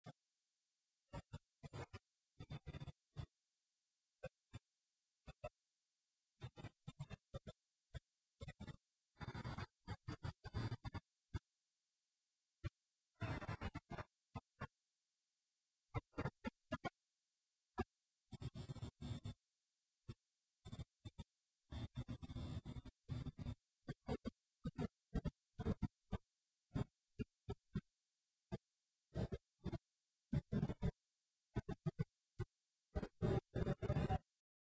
News 1st "News Car Vandalized"
U.S. Citizen WARNING!
2012 Media Subversion List - Coup D'Etat , Treason USA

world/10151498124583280

Part of the Price of being a freelance news person. While covering a medium to light riot, 4 out 4 tires slashed. 3 down, 1 held air. Some of the costs of being in the News Business You will never be compensated for.